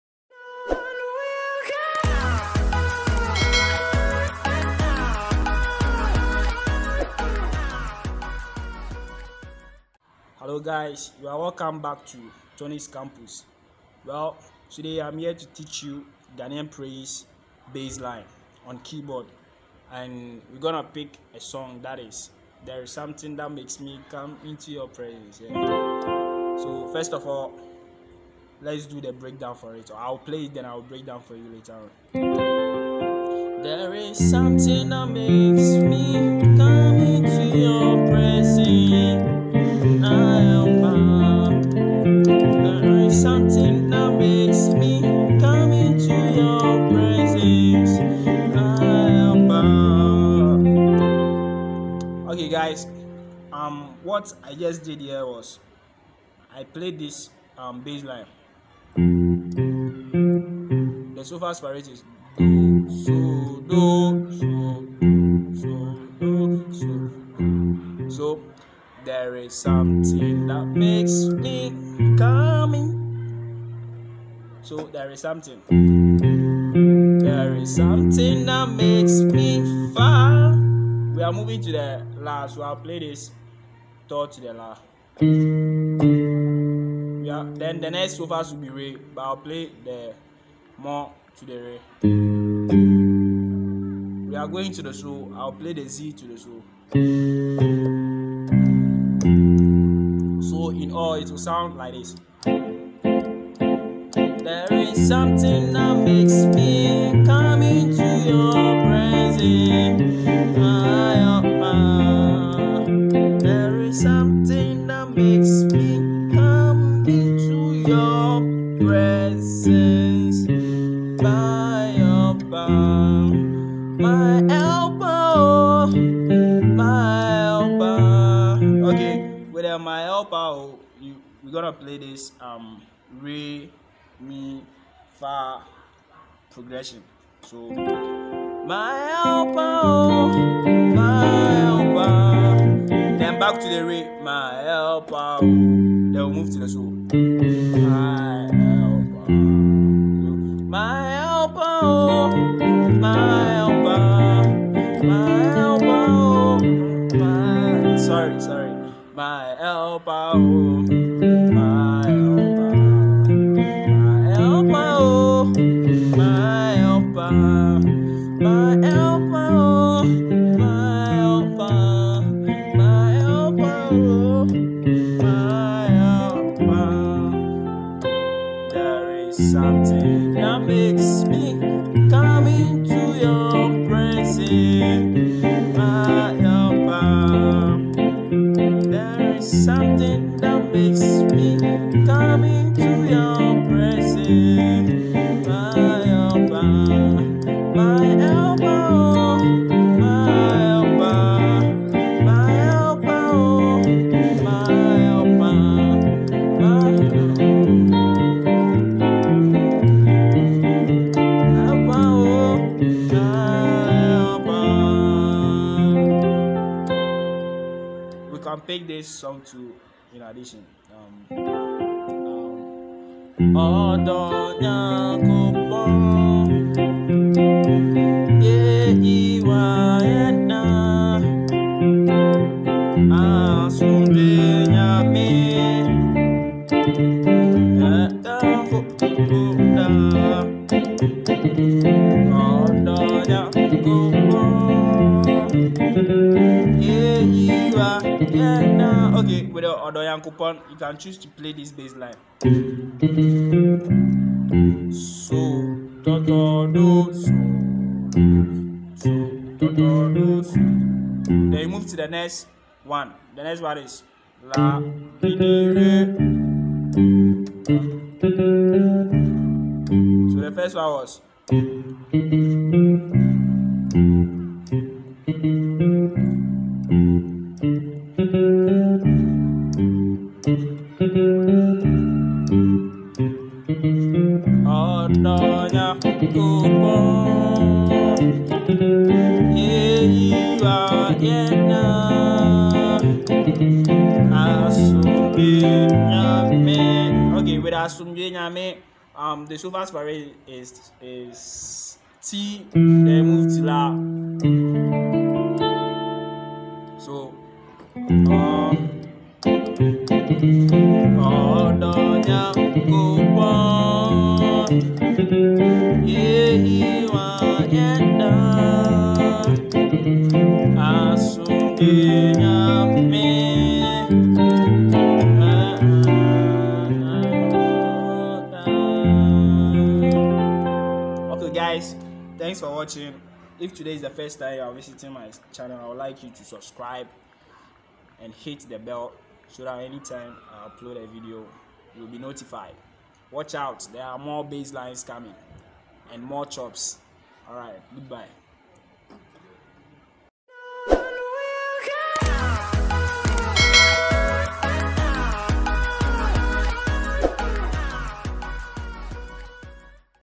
How to play Ghanaian praises on piano lesson#2
Ghanaian praises keyboard bass lines